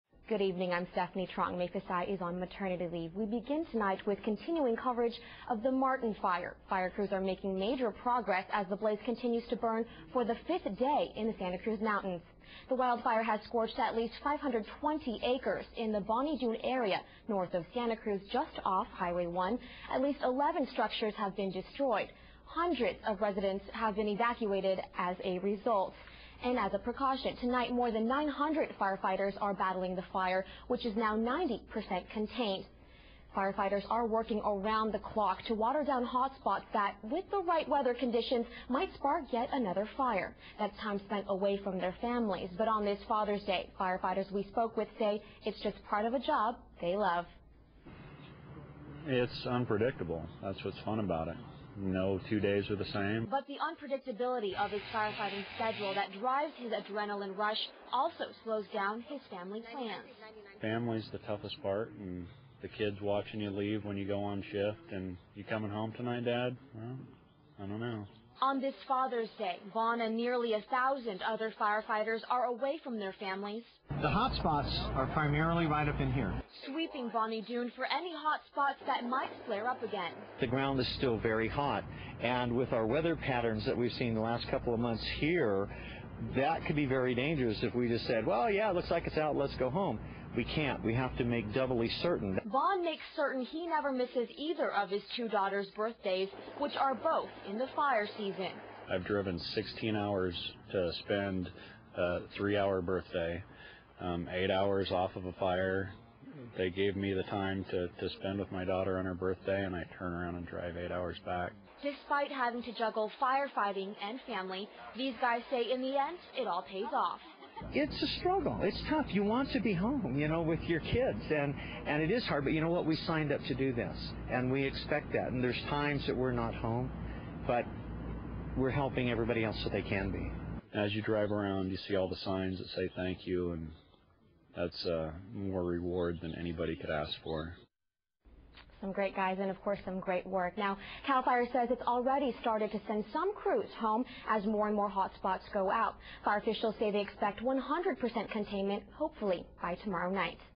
Firefighters Spend Time Away From Families
Nearly a thousand firefighters are sweeping the Martin Fire for hotspots and away from their families.